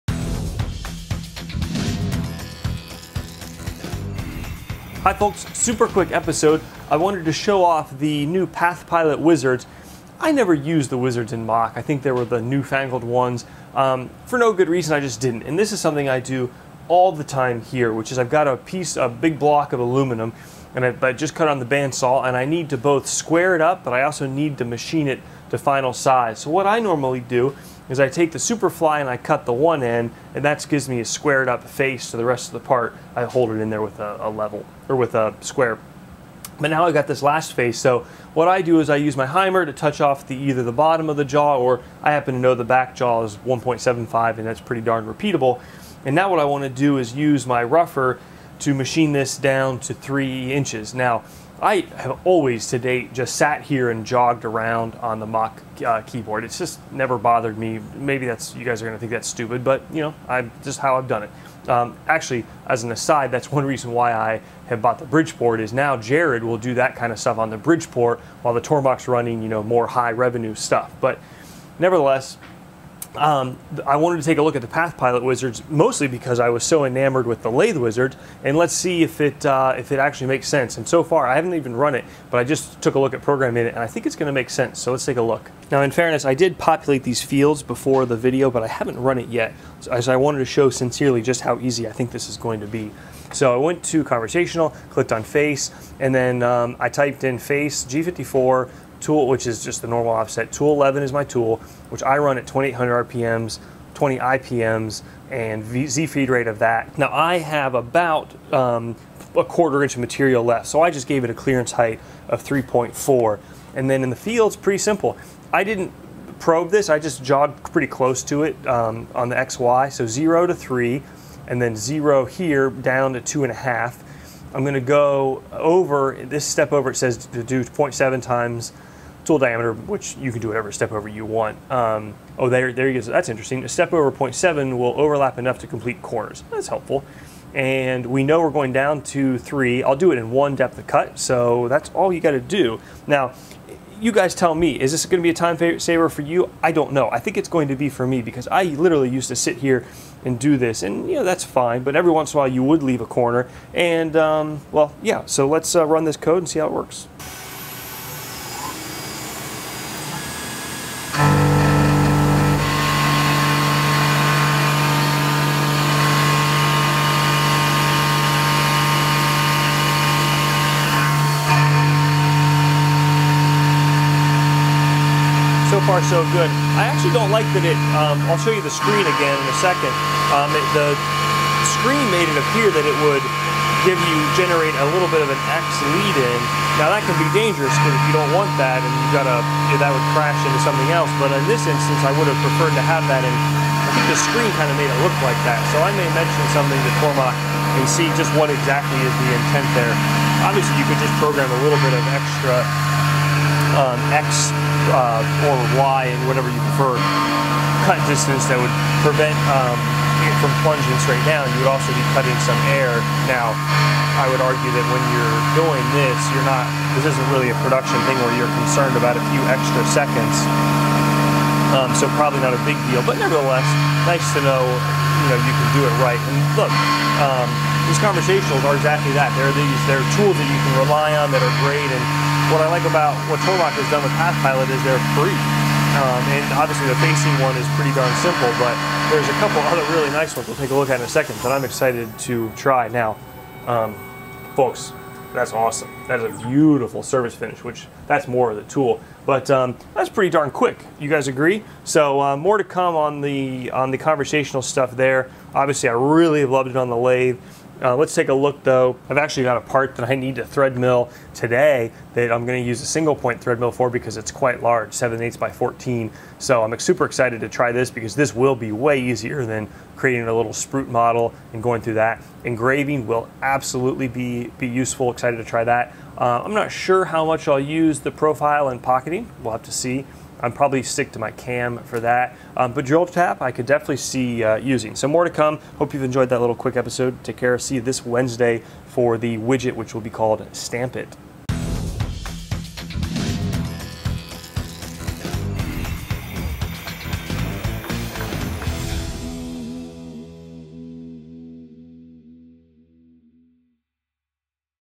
Tormach's PathPilot: Conversational Quick Look!
I often use the keyboard to jog the Tormach PCNC mill to clean up parts, but with the free conversational programs in Tormach's new PathPilot controller, let's give the facing op a test drive!